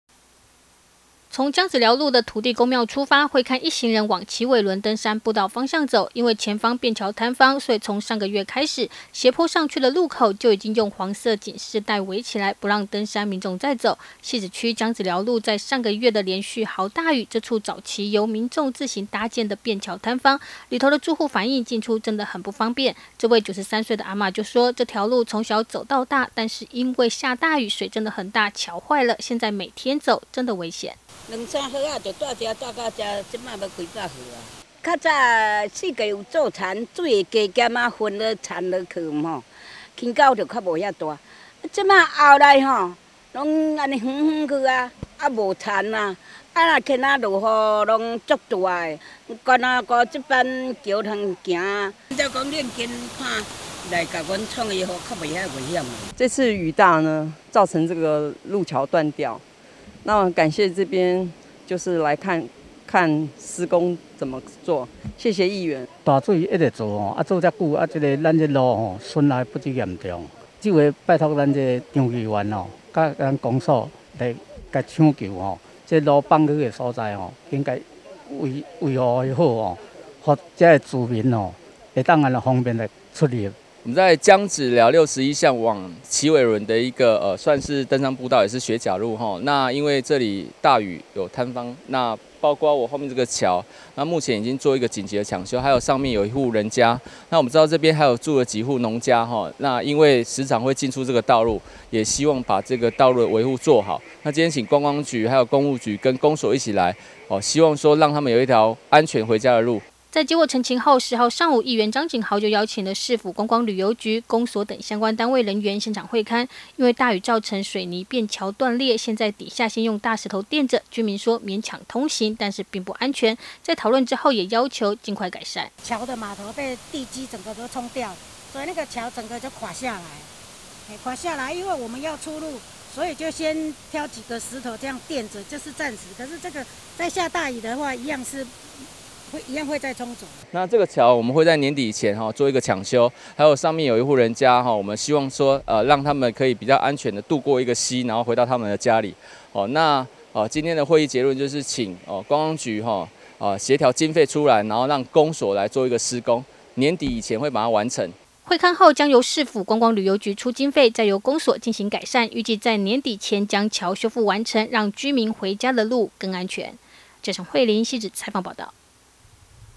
姜子寮路橋坍居民難進出 年底修復便橋
汐止區姜子寮路的一處便道，在上個月的豪大雨便橋坍方，居民抱怨進出不方便也危險，所以，新北市議員張錦豪就再度召集相關單位人員現場會勘，希望能夠儘快搶修，而在討論後也決定年底前修復便橋，讓民眾進出更安全。